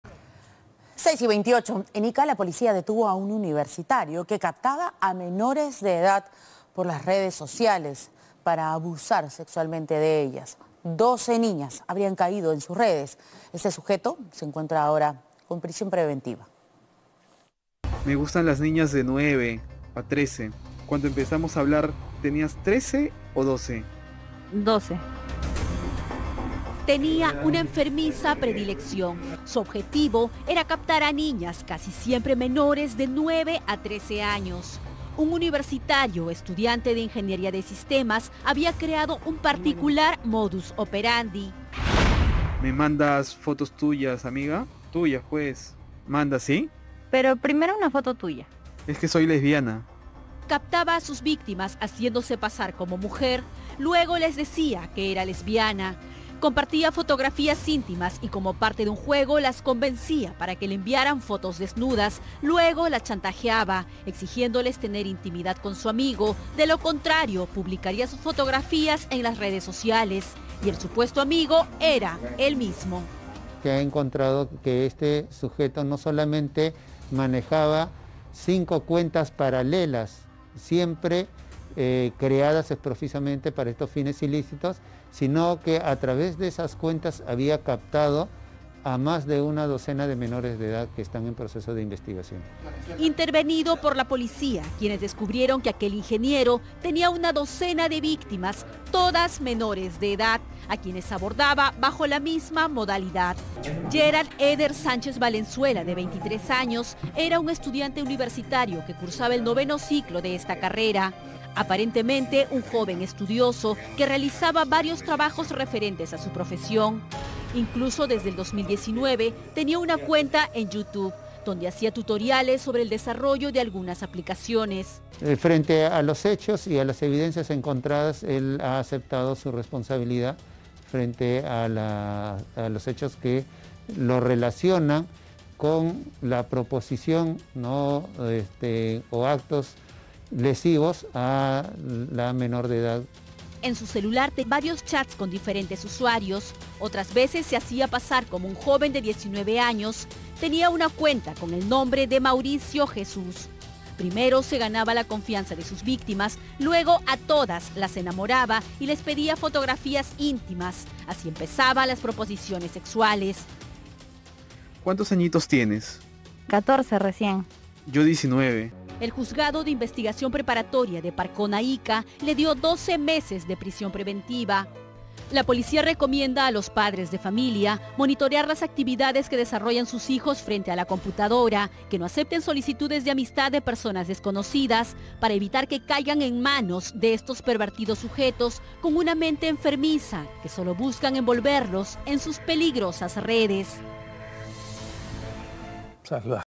Cae sujeto que contactaba a menores de edad por redes sociales | Primera Edición
La investigación reveló que 12 víctimas fueron agredidas por el sujeto, quien fue puesto en prisión preventiva por las autoridades.

Hagámoslo bien.